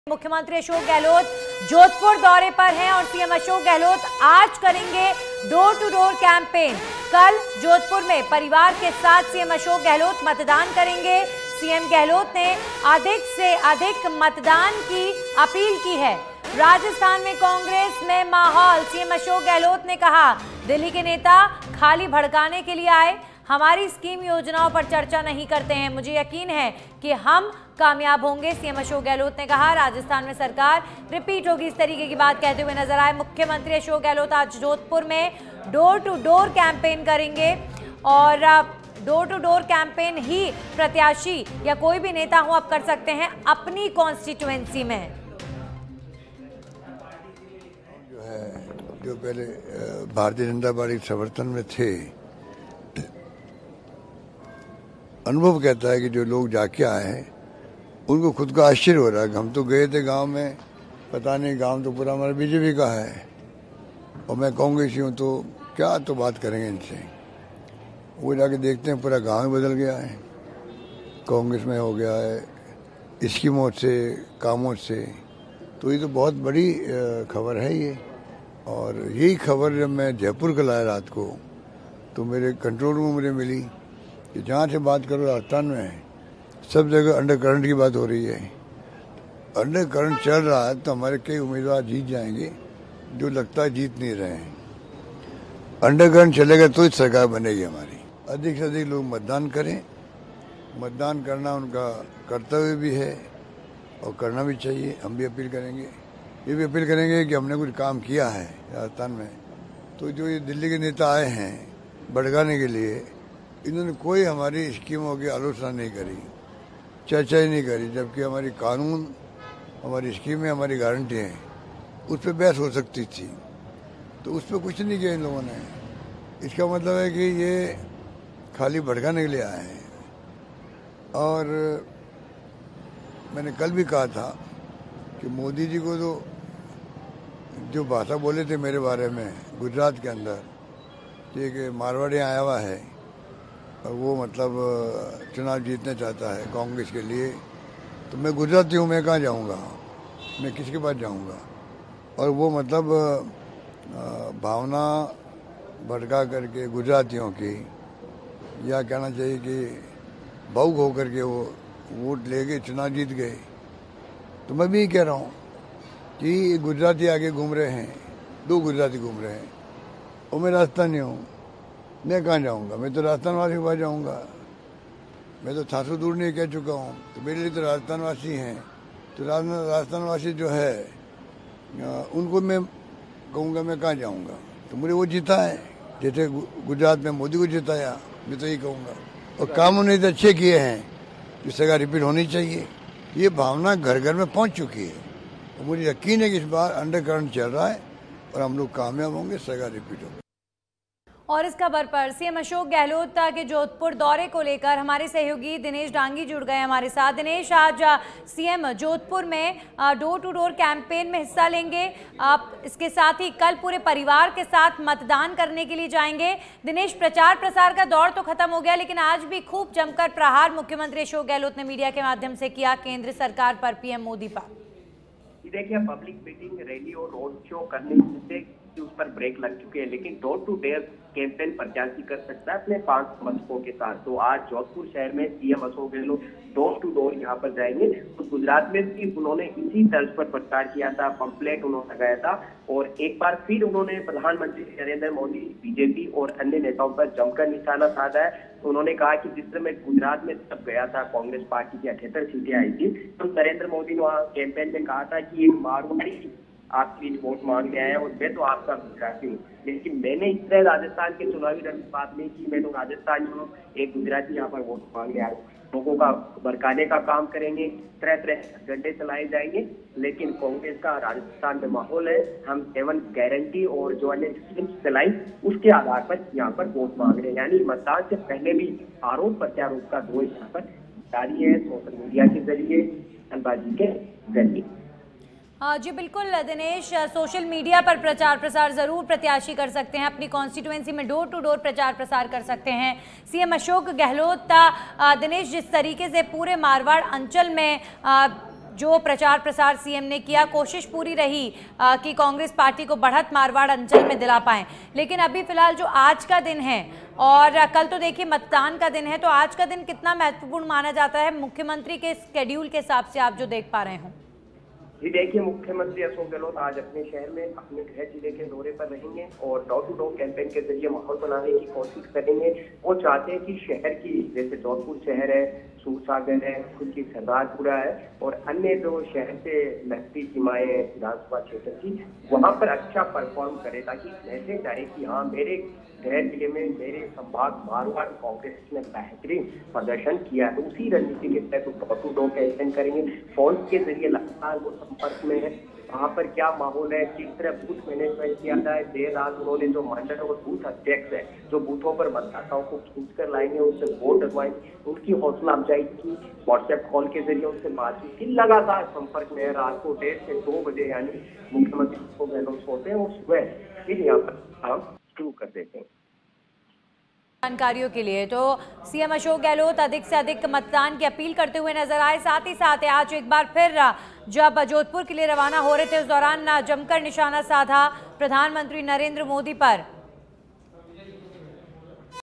राजस्थान में कांग्रेस मय माहौल, दिल्ली वाले नेता कुछ नहीं कर पाएंगे: CM Gehlot | Rajasthan Election
About Video: राजस्थान में कांग्रेस मय माहौल, दिल्ली वाले नेता कुछ नहीं कर पाएंगे: CM Gehlot | Rajasthan Election